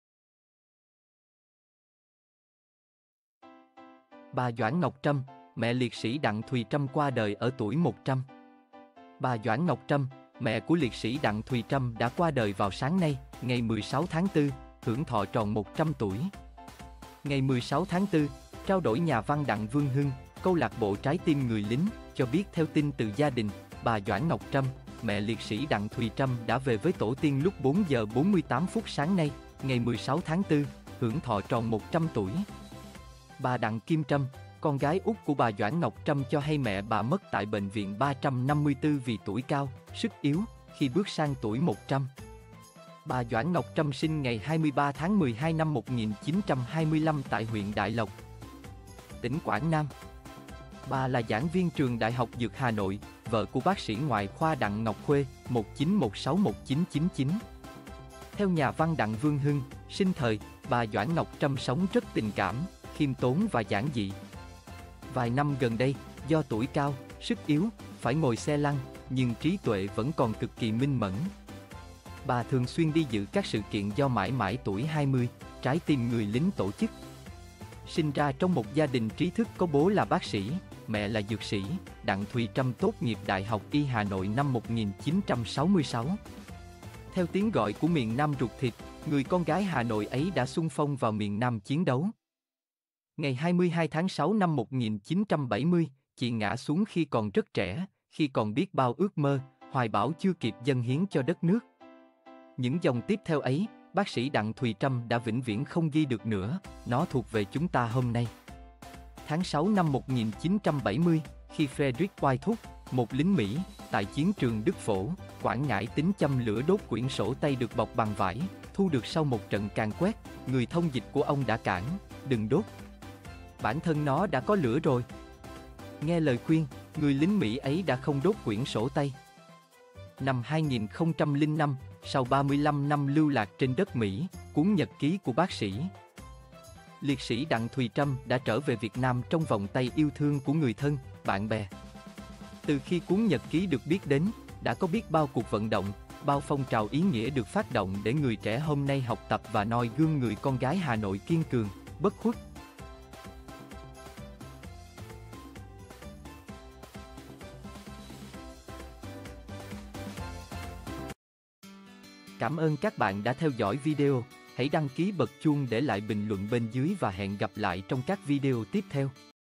Bà Doãn Ngọc Trâm, mẹ liệt sĩ Đặng Thùy Trâm qua đời ở tuổi 100
Bà Doãn Ngọc Trâm, mẹ của  liệt sĩ Đặng Thùy Trâm đã qua đời vào sáng nay (ngày 16/4), hưởng thọ tròn 100 tuổi.

Ngày 16-4, trao đổi  nhà văn Đặng Vương Hưng (Câu lạc bộ Trái Tim người lính) cho biết theo tin từ gia đình, bà Doãn Ngọc Trâm, mẹ liệt sĩ Đặng Thùy Trâm đã về với tổ tiên lúc 4 giờ 48 phút sáng nay, ngày 16/4, hưởng thọ tròn 100 tuổi.

Bà Đặng Kim Trâm, con gái út của bà Doãn Ngọc Trâm cho hay mẹ bà mất tại Bệnh viện 354 vì tuổi cao, sức yếu, khi bước sang tuổi 100.

Bà Doãn Ngọc Trâm sinh ngày 23/12/1925 tại huyện Đại Lộc, tỉnh Quảng Nam. Bà là giảng viên Trường đại học Dược Hà Nội, vợ của bác sĩ ngoại khoa Đặng Ngọc Khuê (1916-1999).

Theo nhà văn Đặng Vương Hưng, sinh thời, bà Doãn Ngọc Trâm sống rất tình cảm, khiêm tốn và giản dị. Vài năm gần đây, do tuổi cao, sức yếu, phải ngồi xe lăn, nhưng trí tuệ vẫn còn cực kỳ minh mẫn. Bà thường xuyên đi dự các sự kiện do Mãi mãi tuổi 20, Trái tim người lính tổ chức.
Sinh ra trong một gia đình trí thức có bố là bác sĩ, mẹ là dược sỹ, Đặng Thùy Trâm tốt nghiệp Đại học Y Hà Nội năm 1966.

Theo tiếng gọi của miền Nam ruột thịt, người con gái Hà Nội ấy đã xung phong vào miền Nam chiến đấu. Ngày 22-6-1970, chị ngã xuống khi còn rất trẻ, khi còn biết bao ước mơ, hoài bão chưa kịp dâng hiến cho đất nước. Những dòng tiếp theo ấy, bác sĩ Đặng Thuỳ Trâm đã vĩnh viễn không ghi được nữa, nó thuộc về chúng ta hôm nay.

Tháng 6-1970, khi Fredric Whitehurst, một lính Mỹ, tại chiến trường Đức Phổ, Quảng Ngãi tính châm lửa đốt quyển sổ tay được bọc bằng vải, thu được sau một trận càn quét, người thông dịch của ông đã cản: "Đừng đốt. Bản thân nó đã có lửa rồi!". Nghe lời khuyên, người lính Mỹ ấy đã không đốt quyển sổ tay.

Năm 2005, sau 35 năm lưu lạc trên đất Mỹ, cuốn nhật ký của bác sĩ, liệt sĩ Đặng Thùy Trâm đã trở về Việt Nam trong vòng tay yêu thương của người thân, bạn bè.

Từ khi cuốn nhật ký được biết đến, đã có biết bao cuộc vận động, bao phong trào ý nghĩa được phát động để người trẻ hôm nay học tập và noi gương người con gái Hà Nội kiên cường, bất khuất.